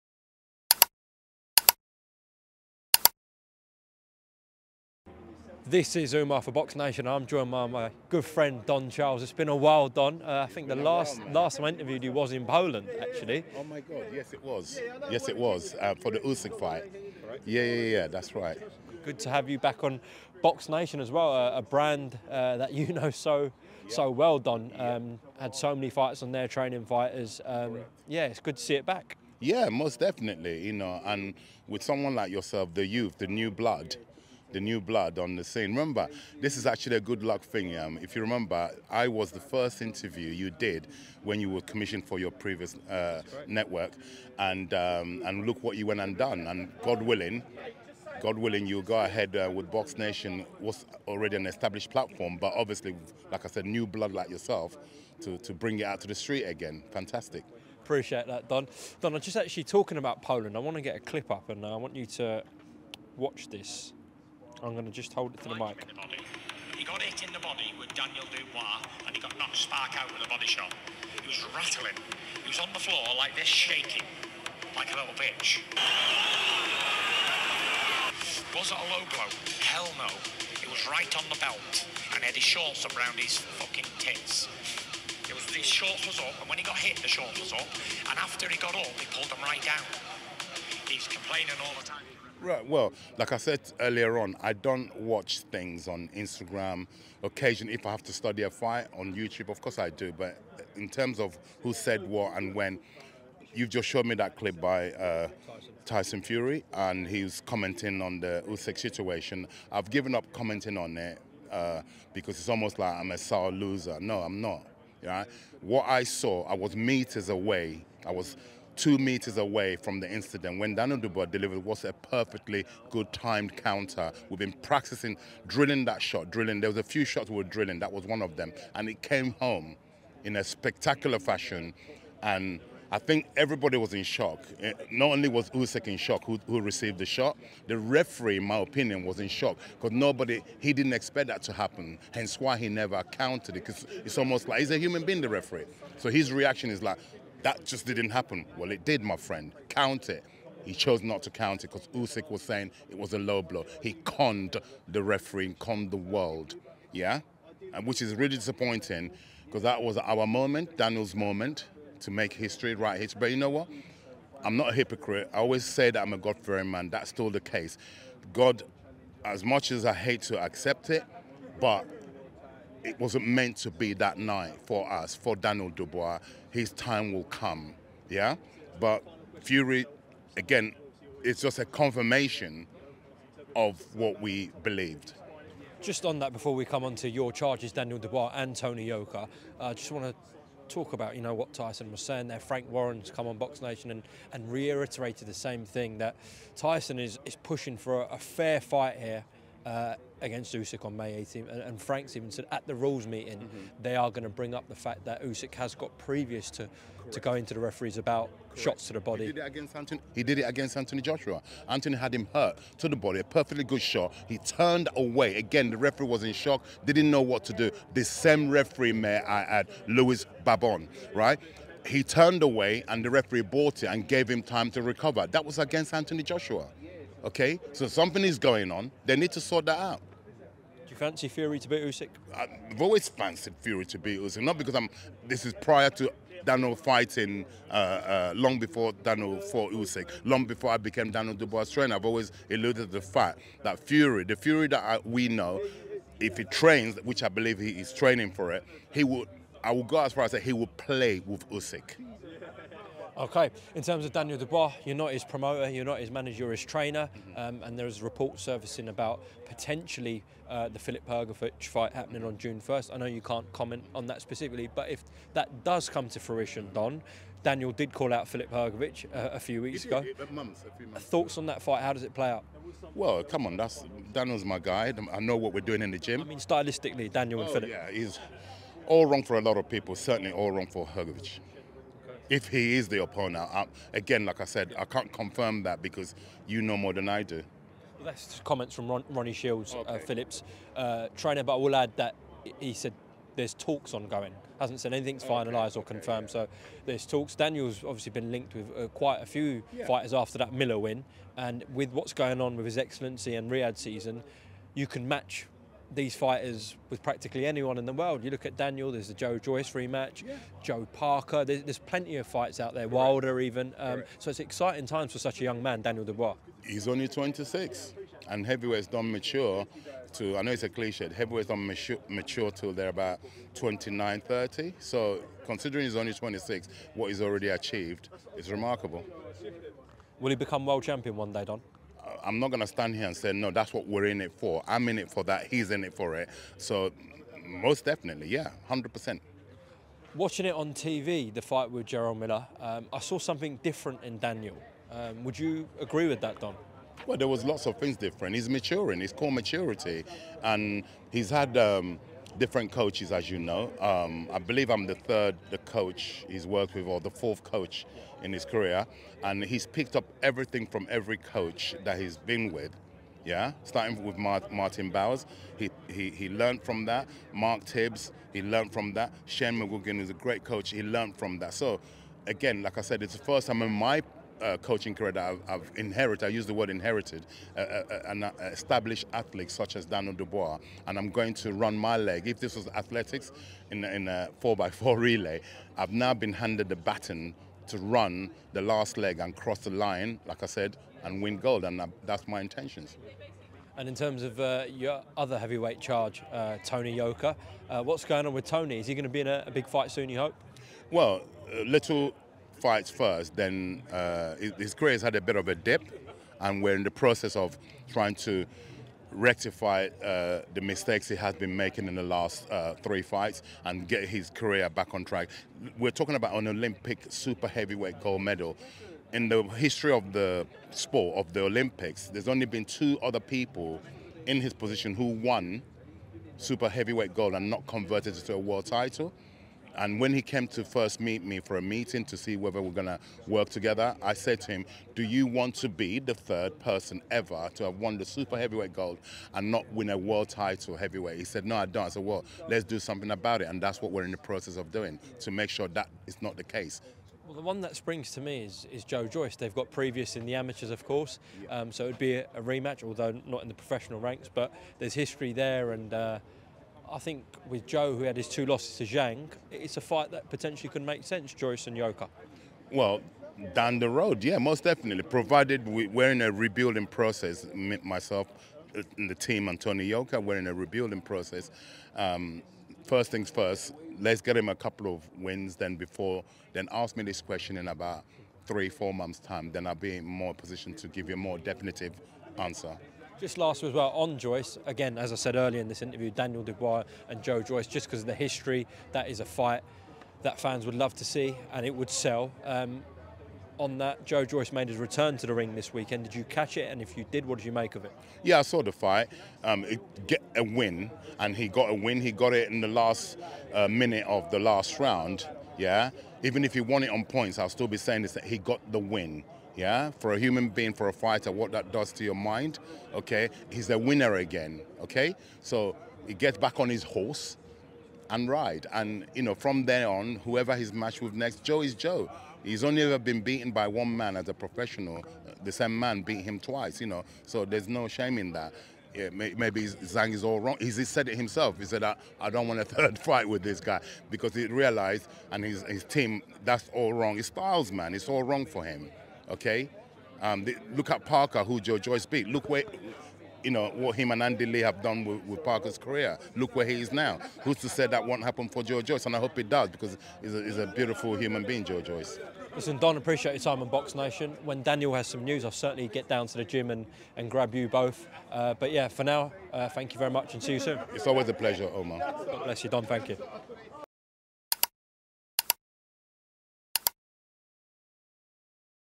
Don Charles EXPLODES IN HEATED RANT about Oleksandr Usyk / says Tyson Fury will "PLAY WITH HIM"
Don Charles speaks to Umar from BoxNation at the opening party for Duffy's boxing elite training centre in East London.